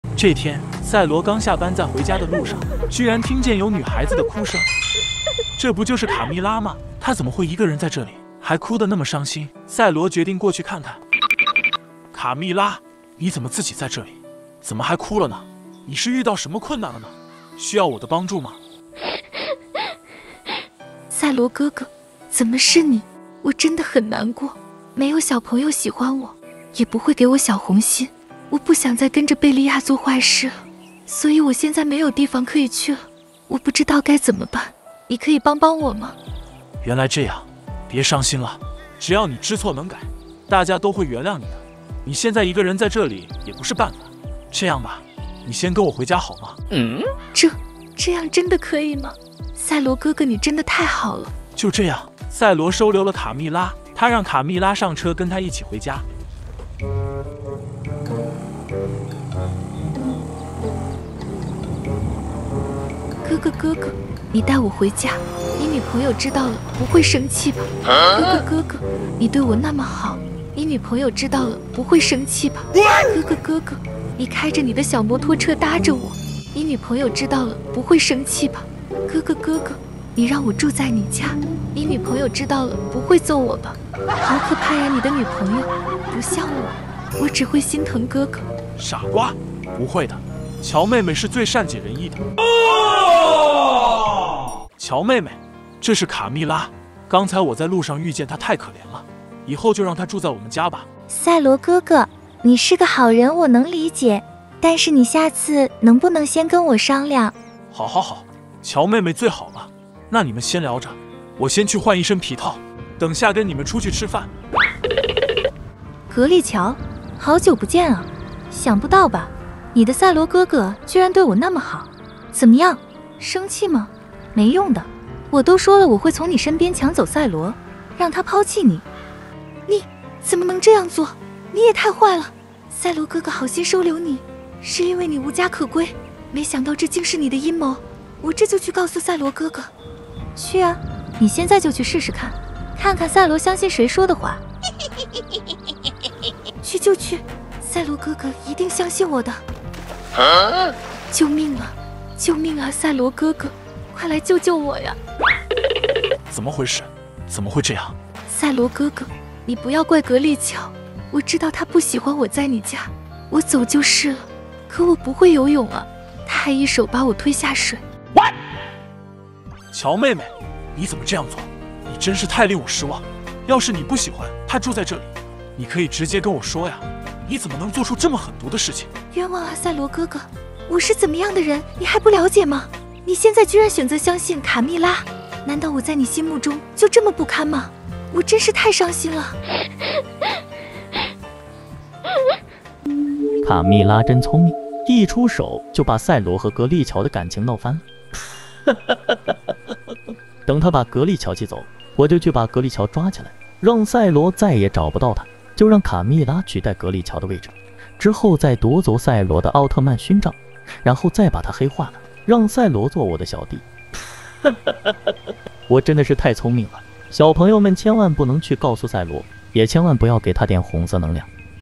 奥特曼 赛罗觉得卡密拉太可怜了，不料确实卡密拉的阴谋
赛罗奥特曼把卡密拉带回家，卡密拉实在是太狡猾了，导致格丽乔被冤枉